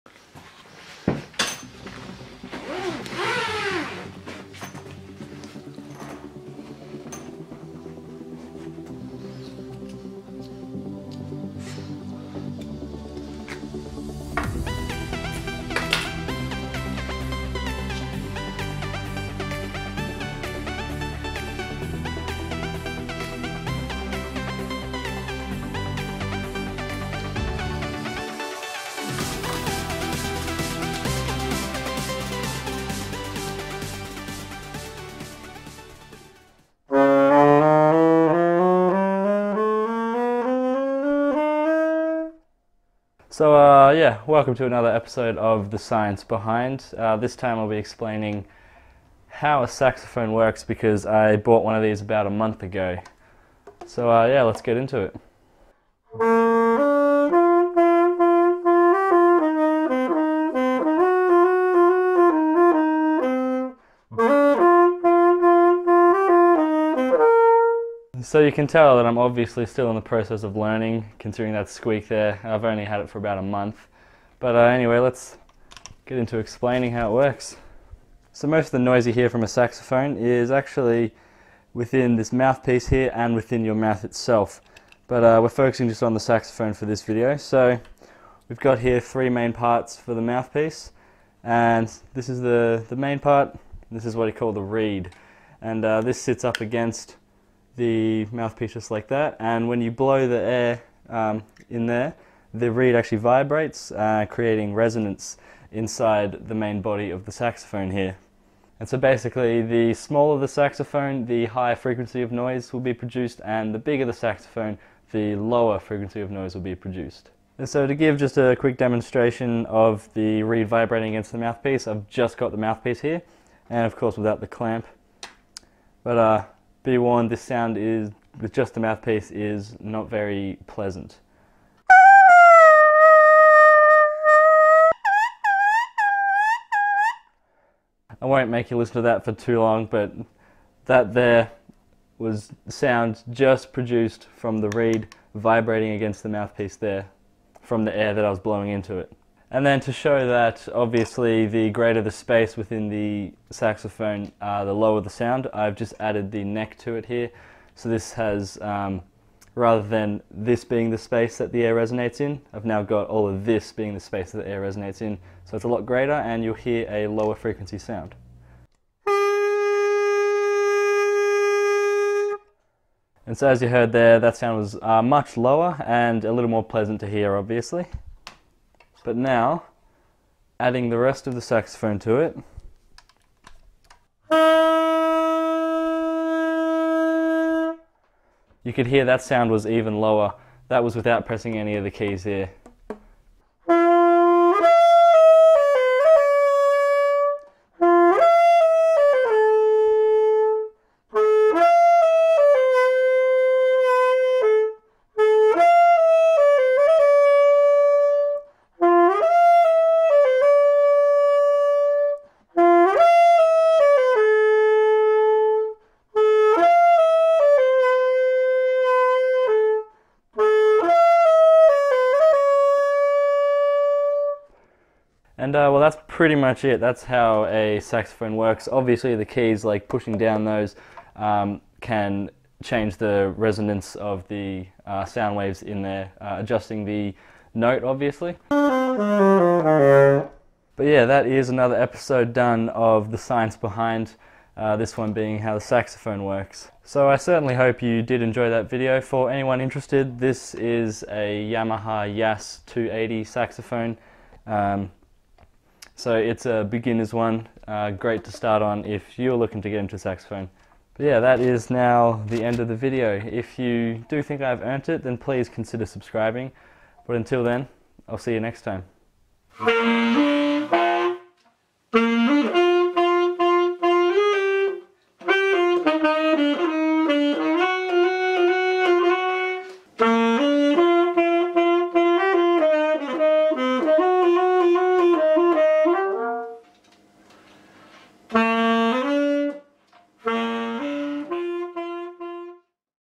How does a saxophone work?
So I recently bought a saxophone and decided to explain how they work for another episode of The Science Behind!

God Bless!